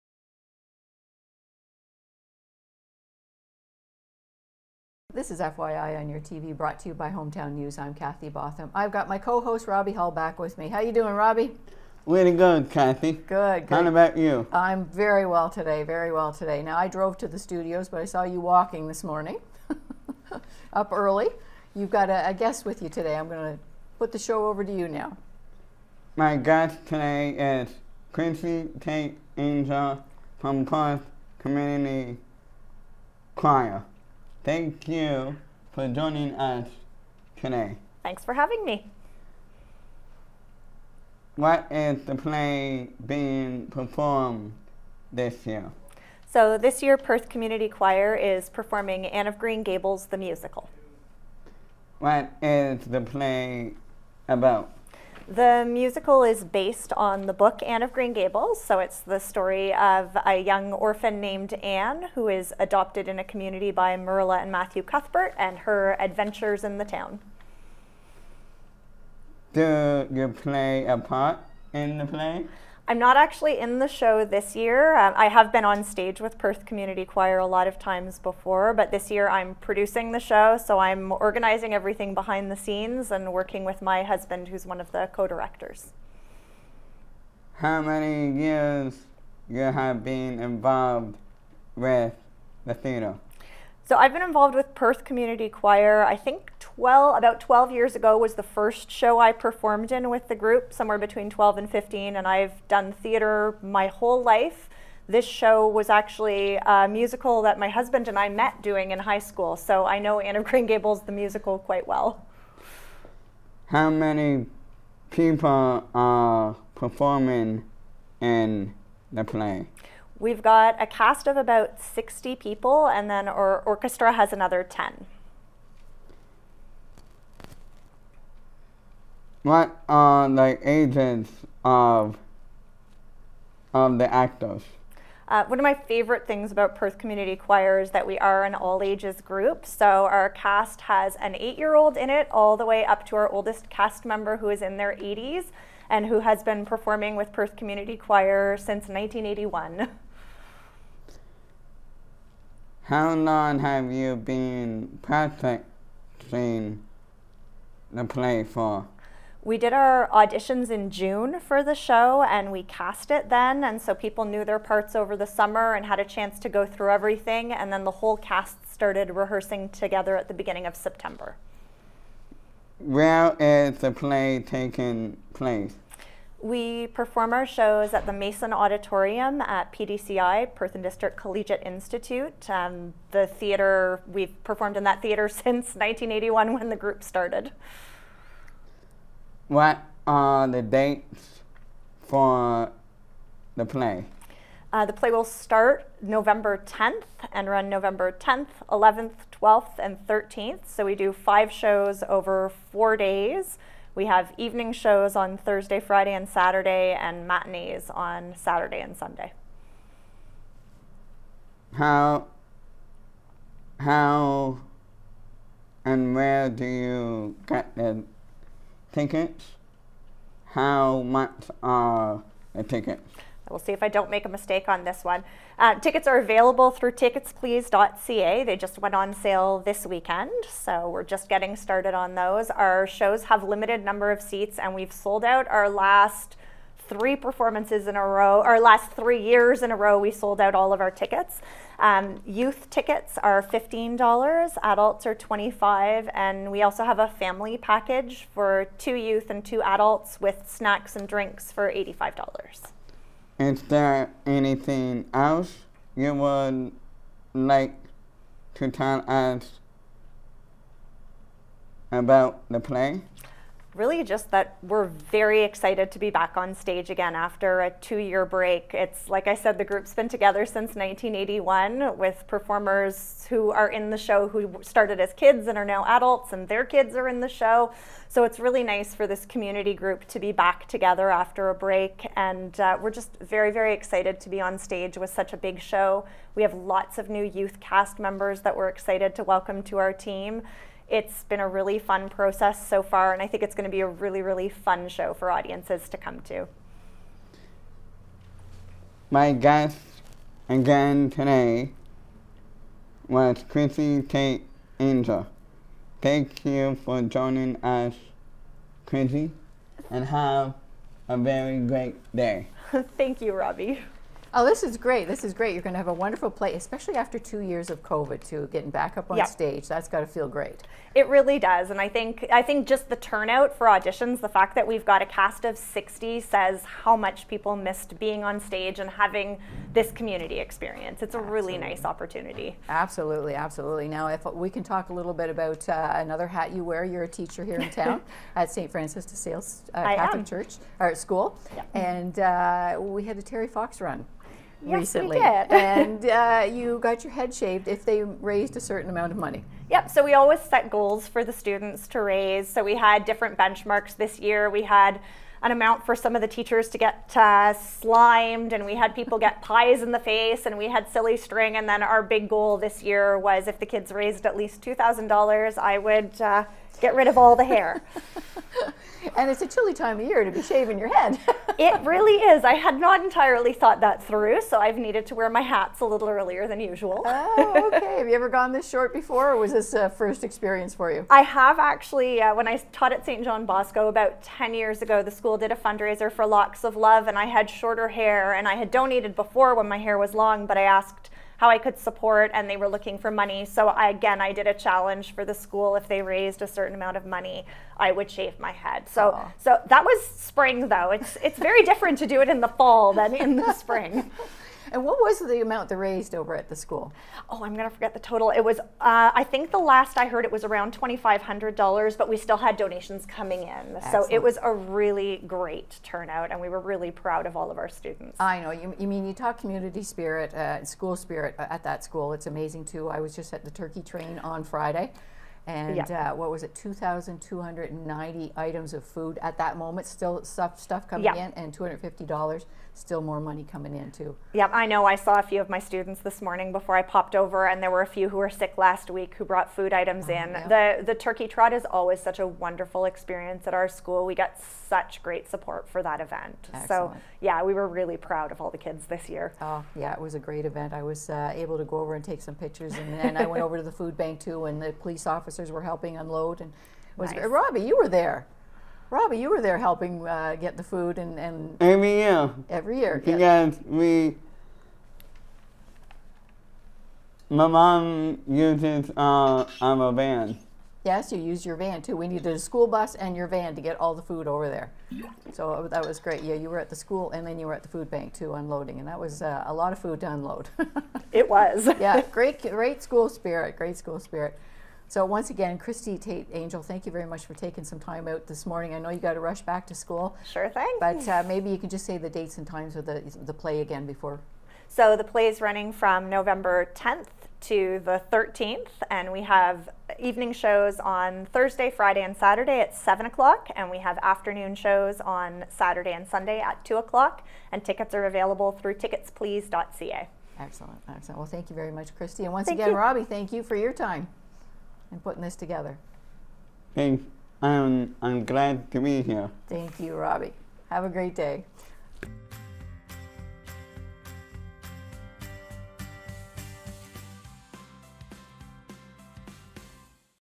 Perth Community Choir
The Perth Community Choir is in action - co-host Robbie Hull finds out more!